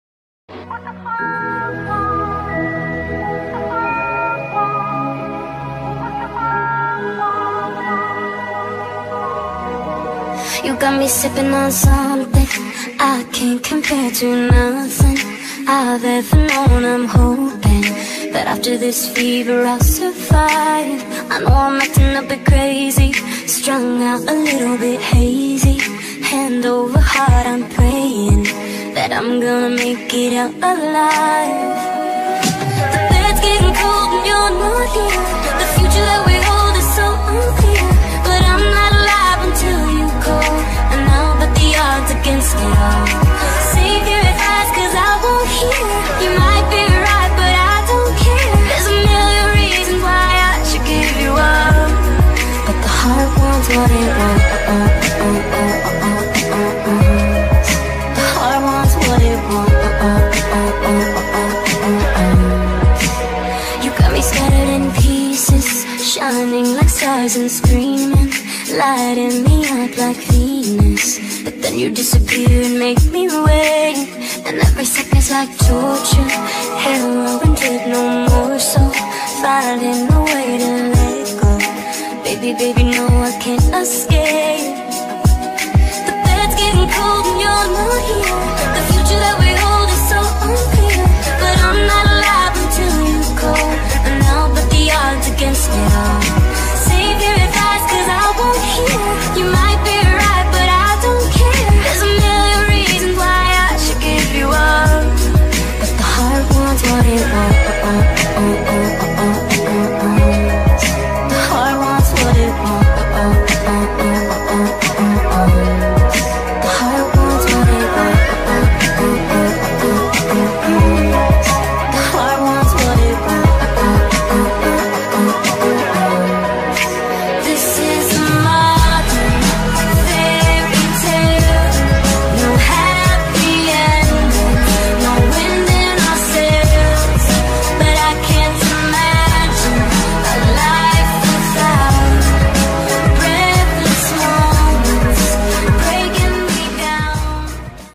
The Love Knot | a wattpad trailer
This video is a trailer for the wattpad novel The Love Knot by Rhuthshana